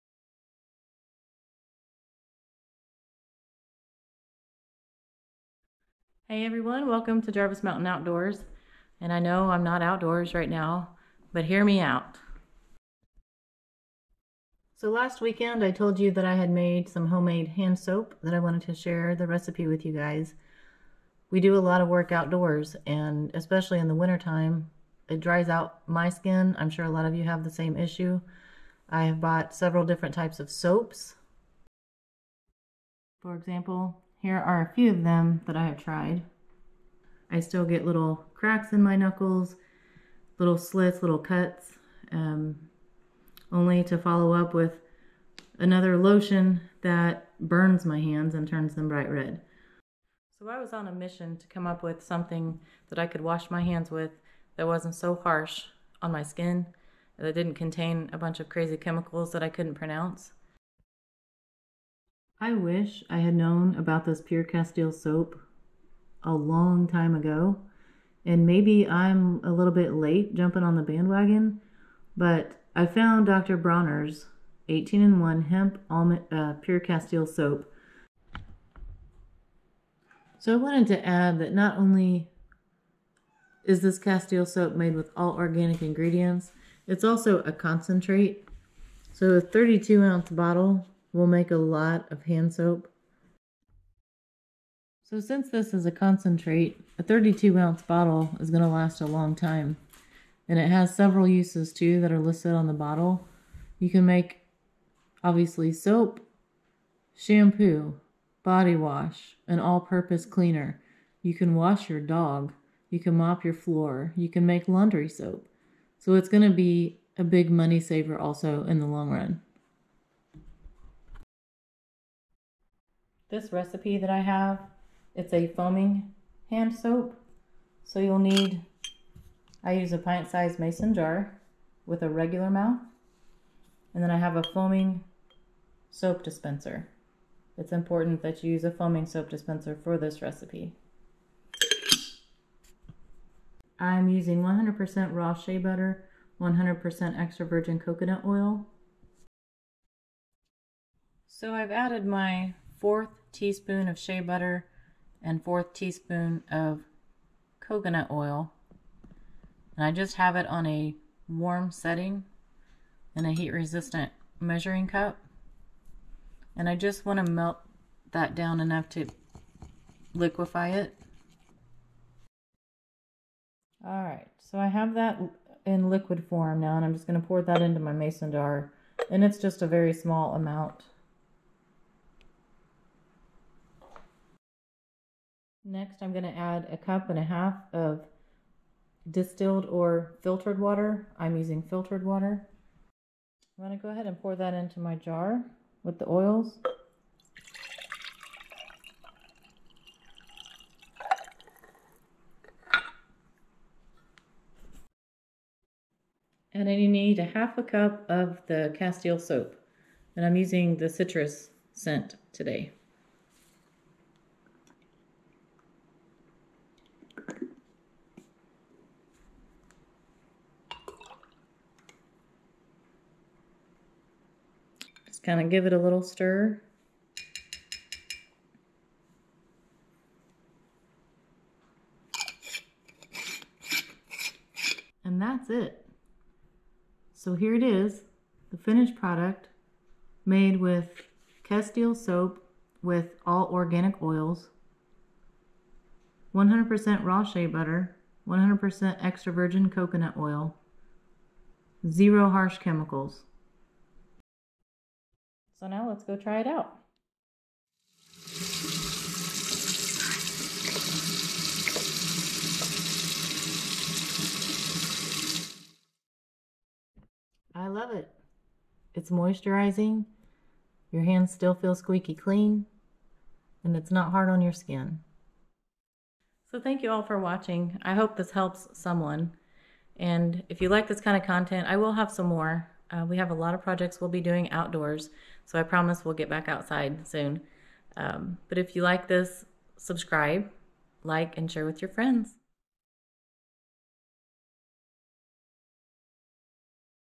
DIY Hand Soap - I wish I had researched this sooner!
I'm on a mission to save my dry, cracked hands!  After some research, this is a simple recipe I made and wanted to share.  I hope this helps someone as much as it has helped me!

Dr Bronner's Castile Soap - Citrus
1/4 tsp coconut oil
1/4 tsp shea butter
1 1/2 cups distilled or filtered water